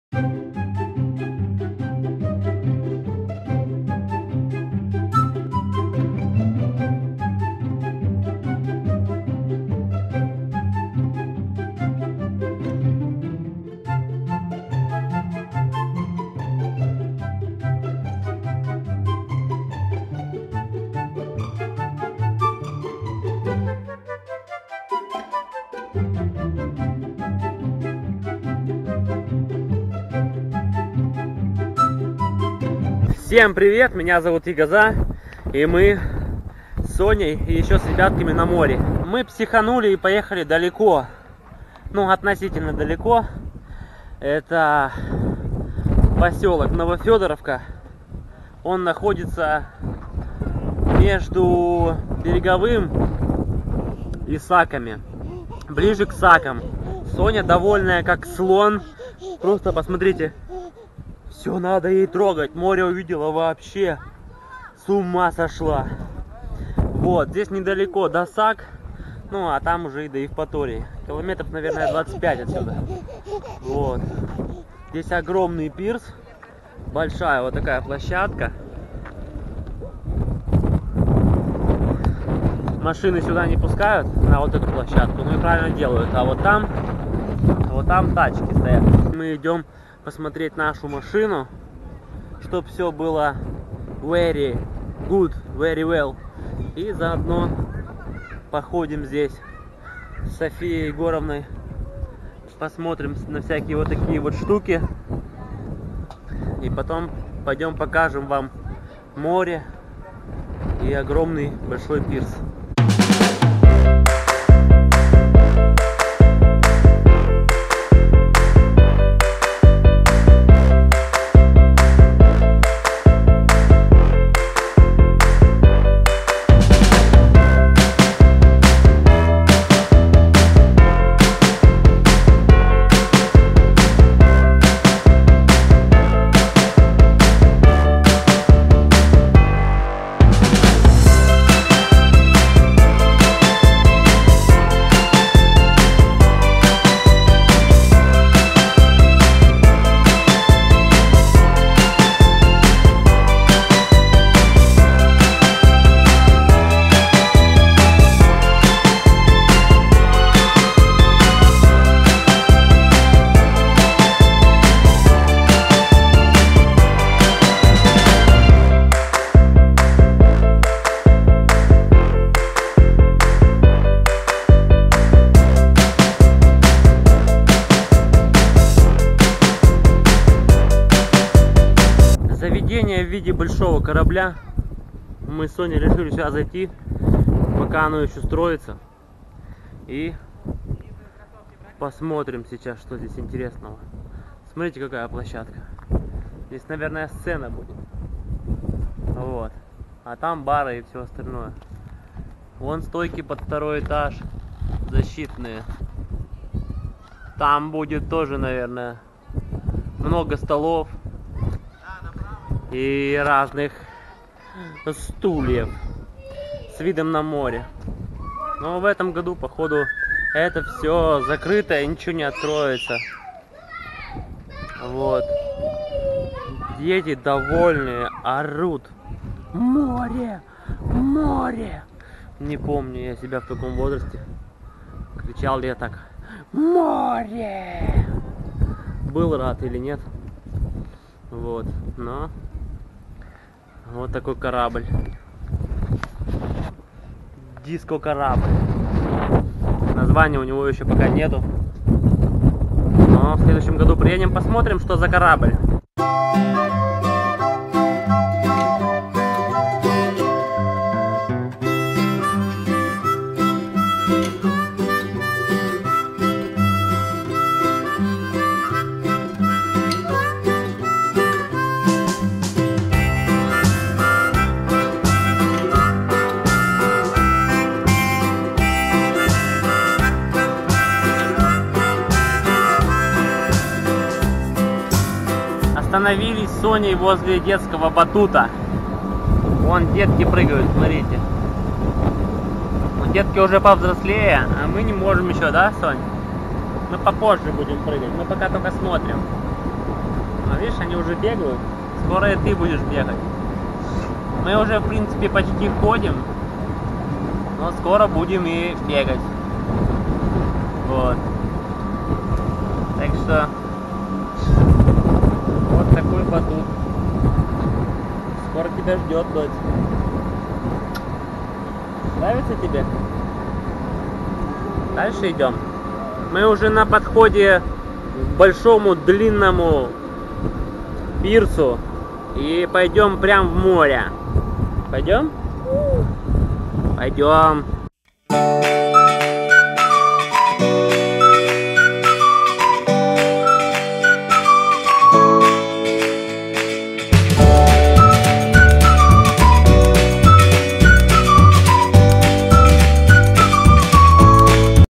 Крым 2018. Новофёдоровка. Диско корабль. Детский батут. Пляж. 1 часть.
Для развития канала,карта РНКБ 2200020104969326 Терюханов Егор Александрович (буду рад любой копейке)